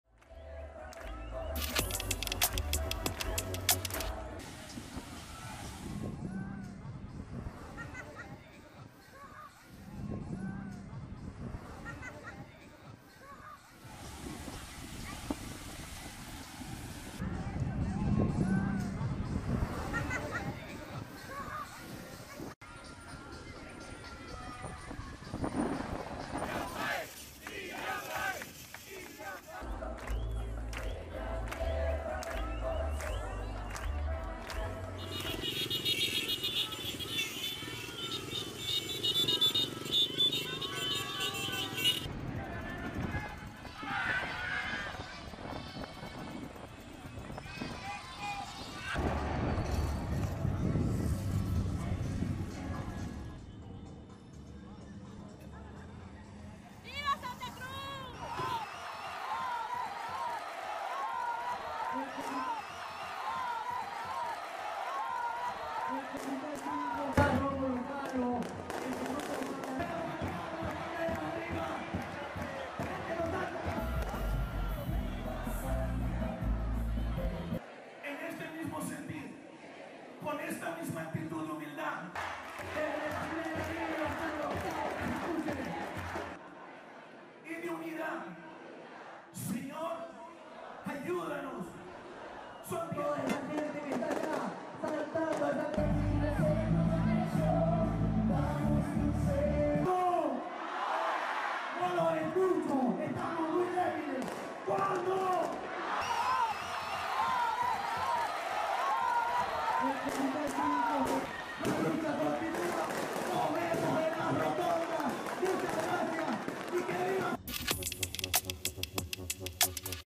شاهد: مظاهرات في مدينة سانتا كروز البوليفية للمطالبة بتقديم موعد إحصاء سكاني…
سانتا كروز التي تعدّ المحرك الاقتصادي للبلاد وتسيطر عليها المعارضة اليمينية كانت بدأت قبل 23 يوماً احتجاجات متواصلة تخللها إغلاق شوارع وطرق، لمطالبة الحكومة اليسارية بإجراء تعداد سكاني مبكر.